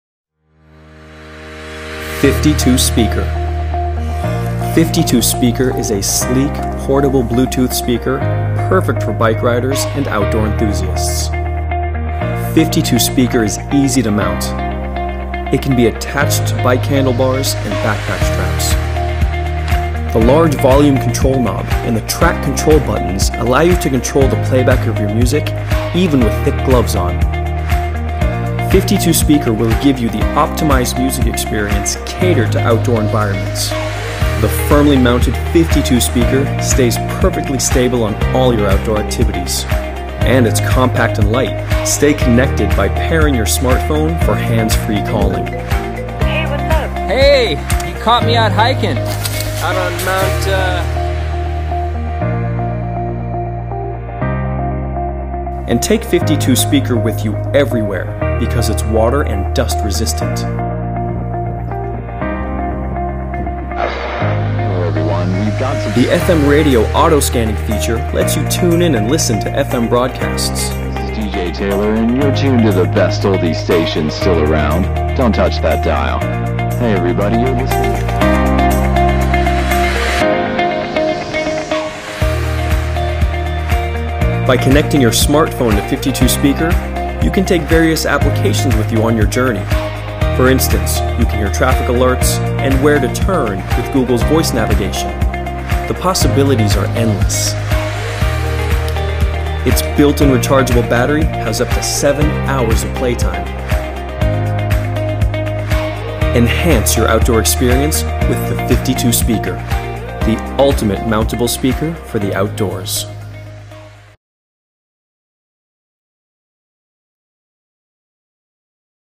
52 SPEAKER - The Ultimate Mountable Speaker for the Outdoors
52 SPEAKER incorporates clear, crisp sound into the timeless design of a whiskey flask. Perfect for on-the-go adventurers and cyclers.